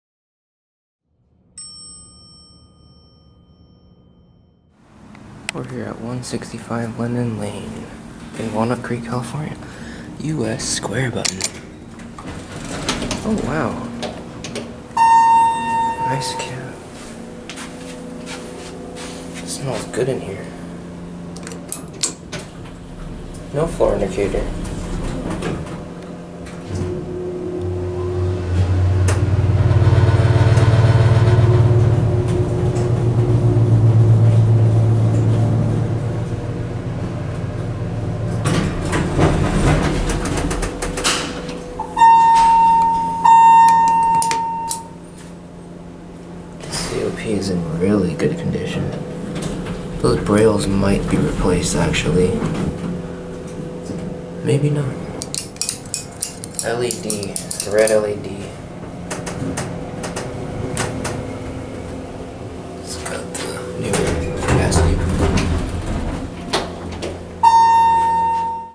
United States Square Button Hydraulic Elevator - 165 Lennon Ln - Walnut Creek, CA
[Recorded 6-9-22] Here's just another basic United States square button in an office building. At this point, I have been on so many of these that they're no longer exciting. However, I will admit that it's always cool to find original ones and I'm lucky to have such an abundance of US spread throughout my area.